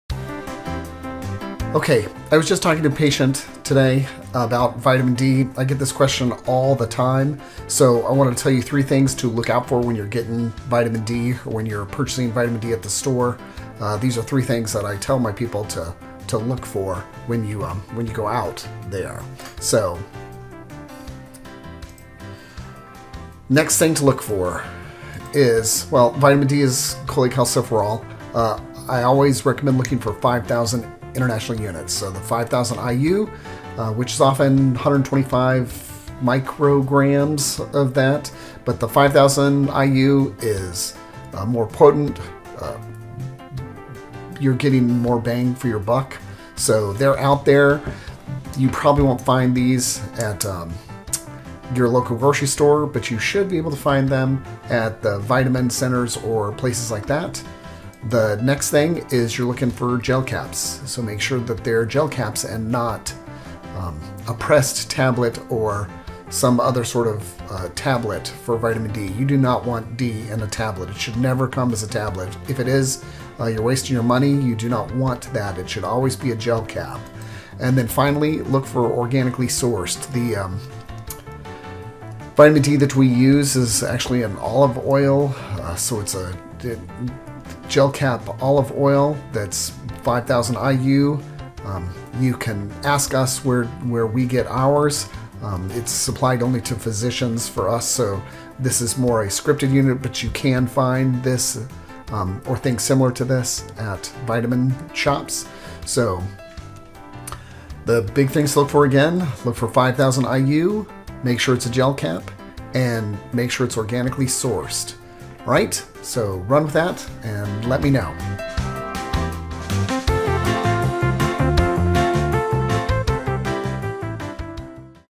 3 Things To Know About Vit D
Here are 3 tips to be aware of before purchasing Vitamin D.  We all know Vit D is helpful in keeping the body's systems functioning correctly, but what amount should we be using and what type is the best for us?  Find out.